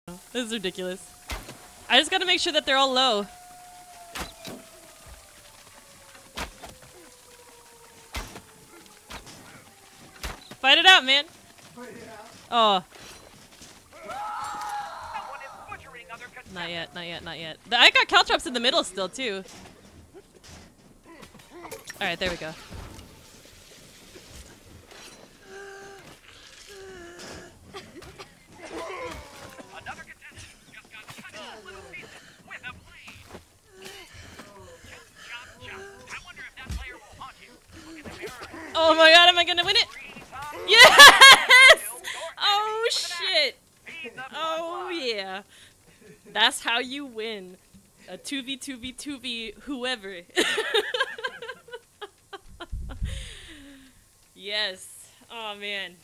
Highlight - 1v2v2
Teams went to bottom to avoid my traps on top. There's no escape for them.